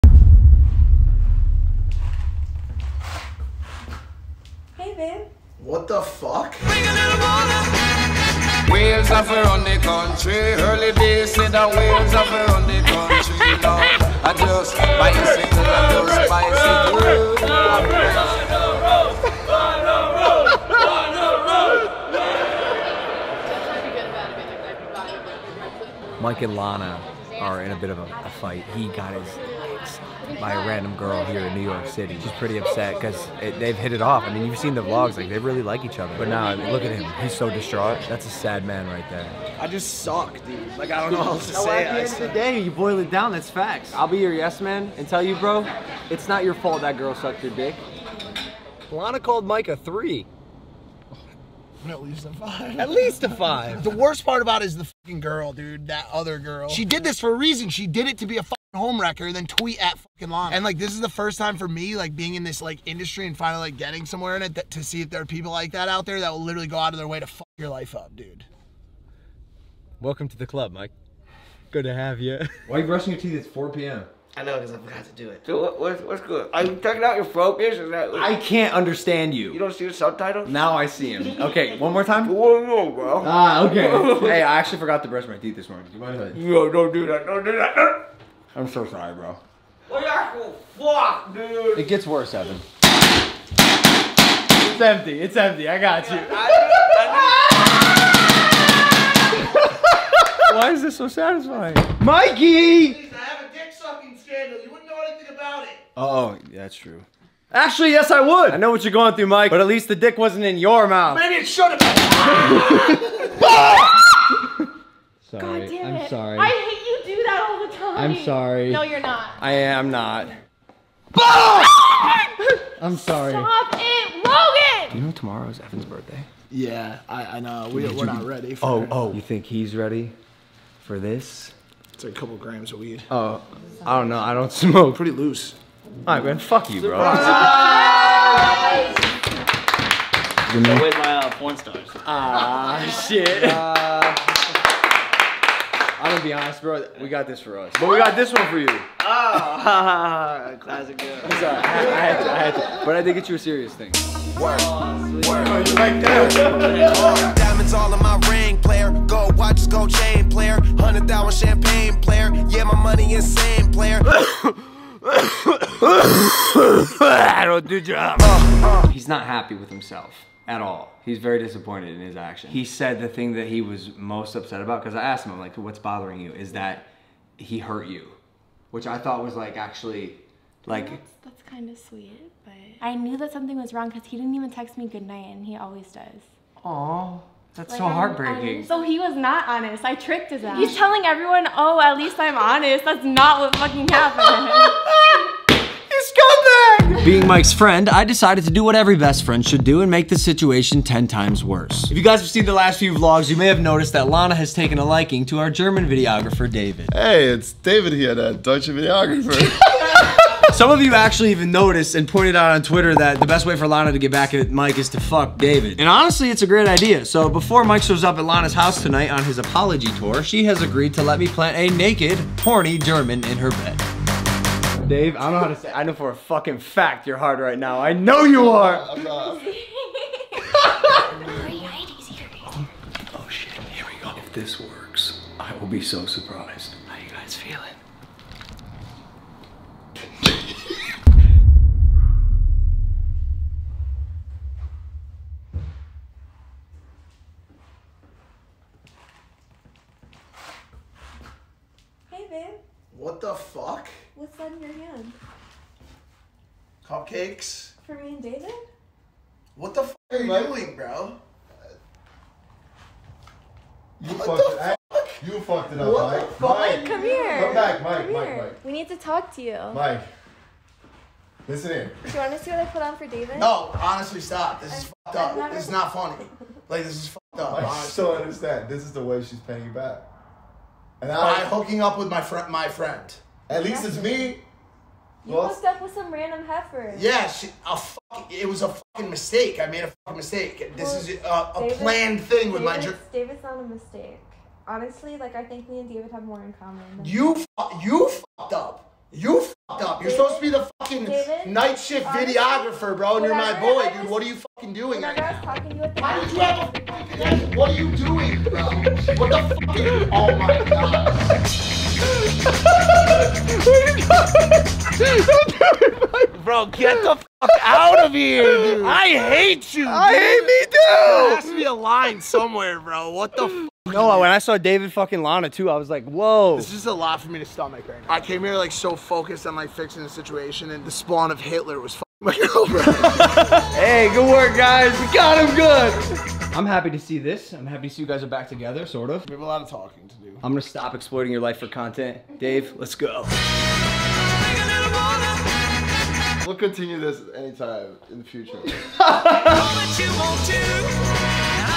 LANA RHOADES GETS REVENGE ON MIKE! (Cheating Prank)
Mike did hanky panky with a girl in New York. His new fling, Lana Rhoades, wasn’t happy about it. I helped make it even :)